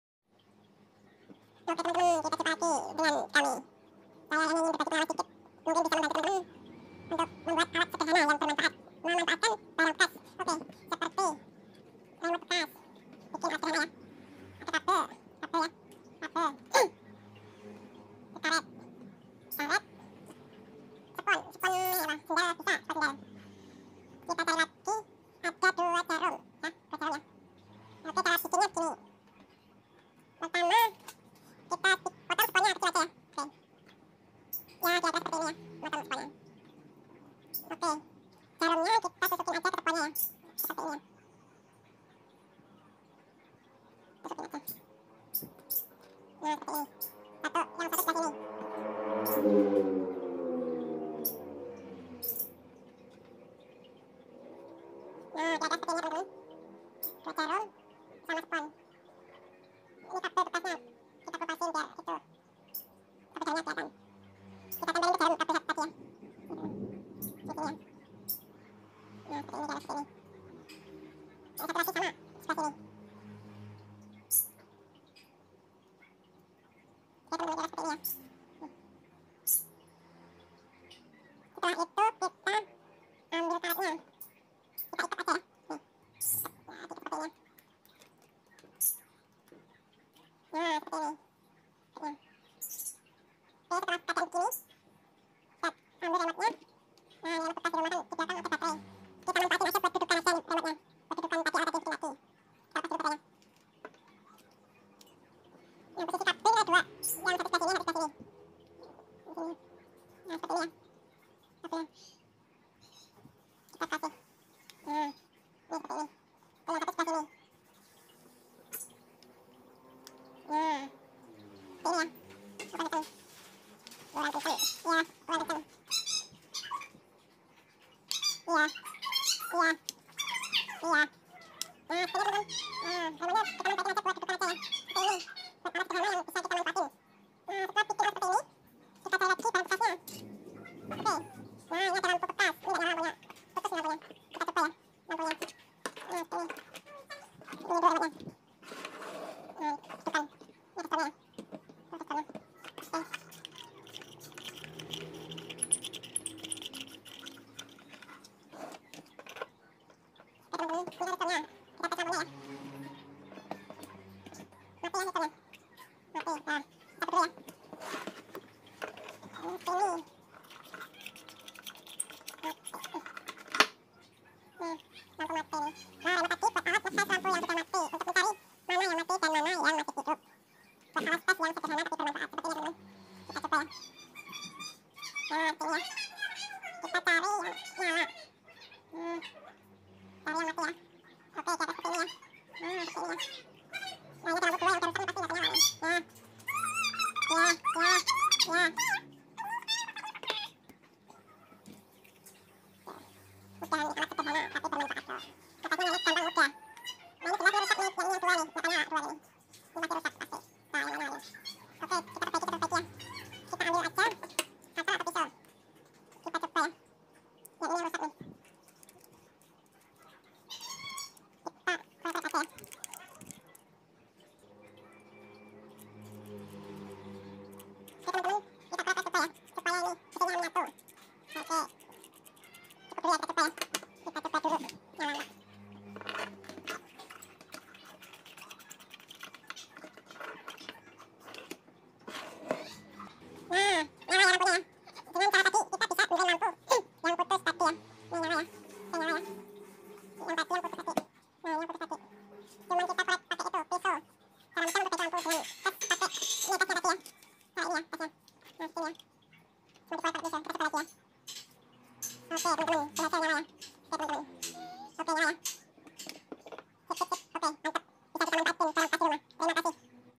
ide kreatif tes lampu dengan alat se adanya 12 Juli 2024
bikin alat untuk tes lampu led